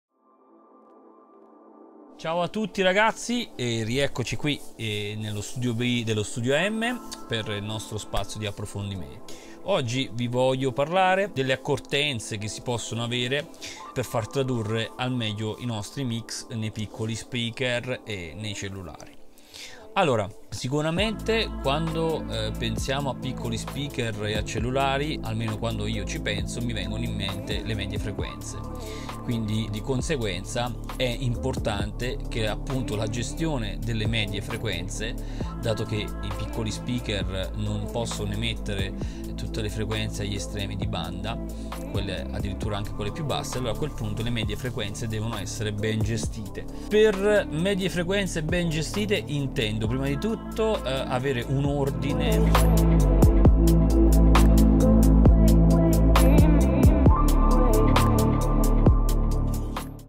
Mixing: come far tradurre al meglio i mix sui piccoli speaker
Salvatore Addeo ci spiega come far tradurre al meglio i mix sui piccoli speaker.